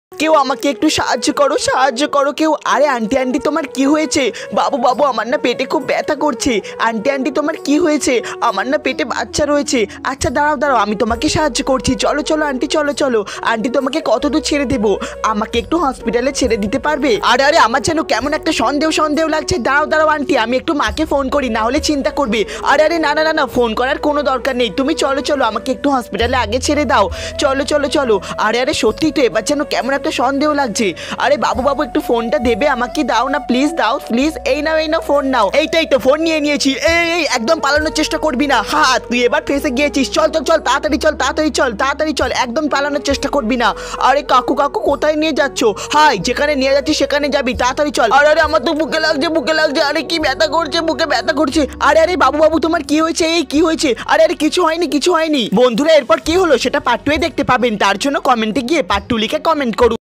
孕婦街頭臨產，小宸哥上前幫忙，沒想到對方竟然是人販子！
❤️傳遞兒童安全知識❤️ 💙我是胡智宸，今年6歲了💙 讓小朋友學會更好的保護好自己~